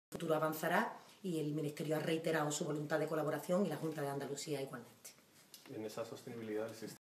Voluntad de colaboración. María Jesús Montero
La consejera de Salud de la Junta de Andalucía, María Jesús Montero, se reúne con la ministra de Sanidad, ana Mato, y le traslada su voluntad de colaboración con el actual gobierno.